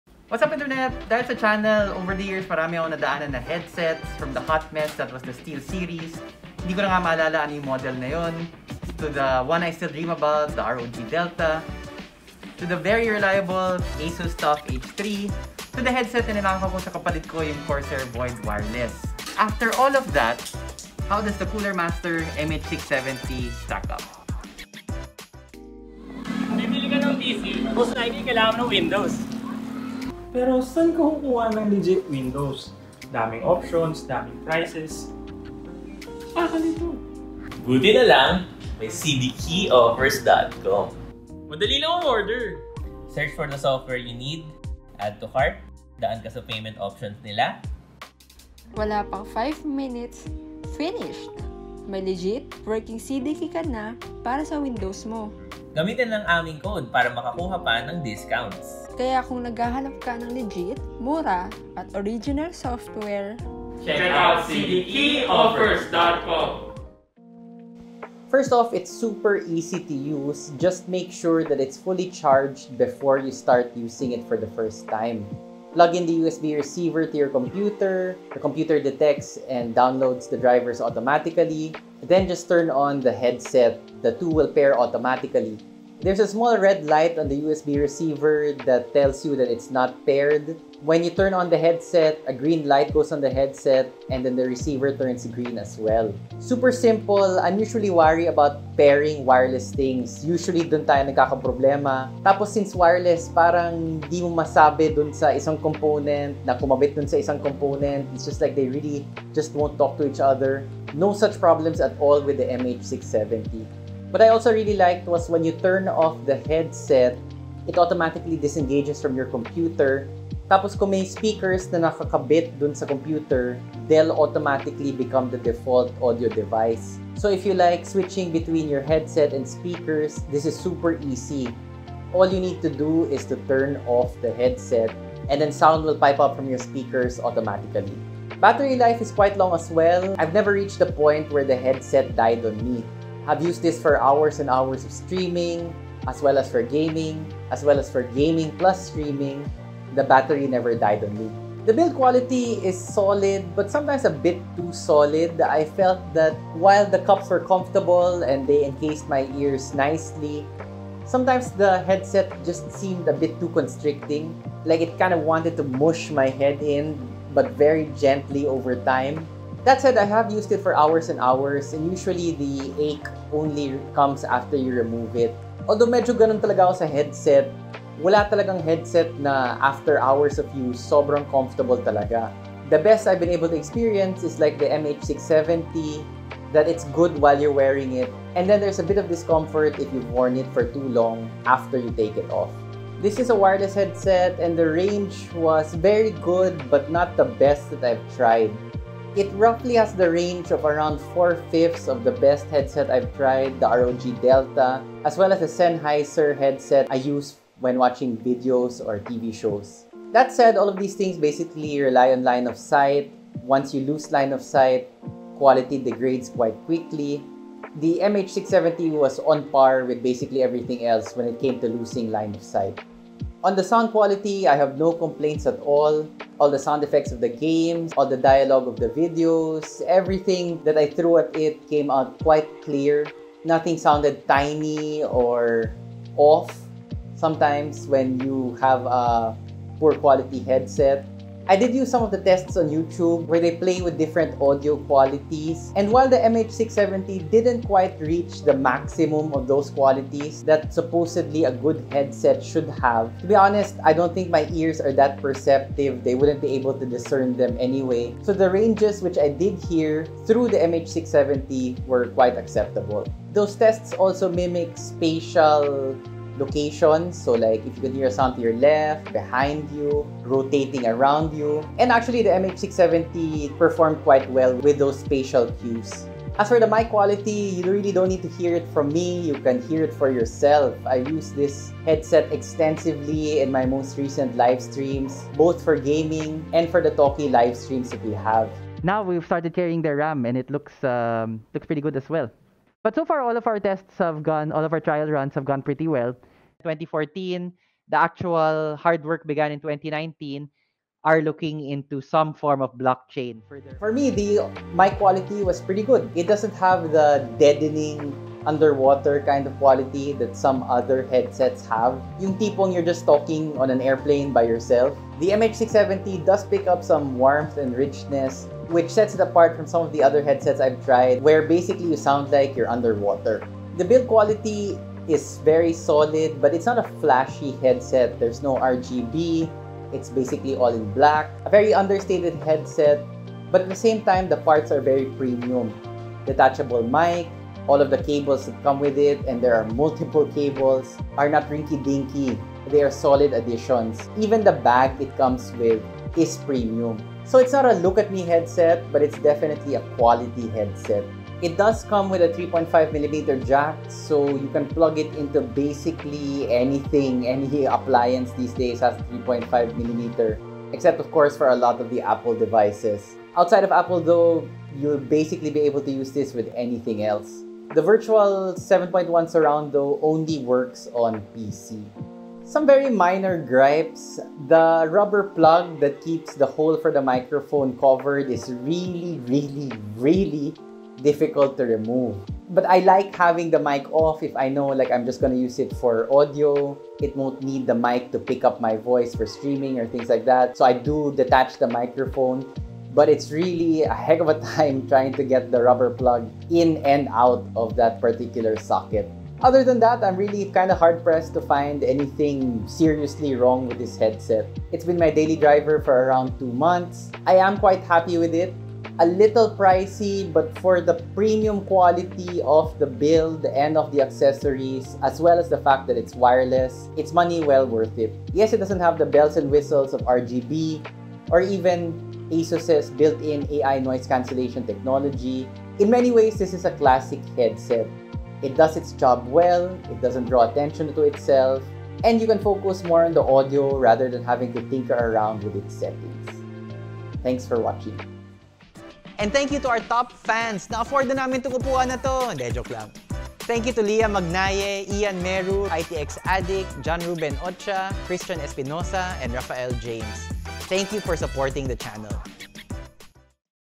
Solid no frills wireless headset 🎙️ CoolerMaster MH670 Review
More Knowledge! 🧐 Videos mentioned in this video

0:00 Start
1:09 Post sponsor
1:36 Easy Wireless
2:19 Battery Life
2:34 Comfort
3:14 Range
3:45 Sound Quality
5:26 Build Quality
6:12 Minor Gripes
6:42 Final Score

FYI on the product: We purchased this item. The brand had no say in the editorial content of this video.

Episode 241x23x22 - MH670 Review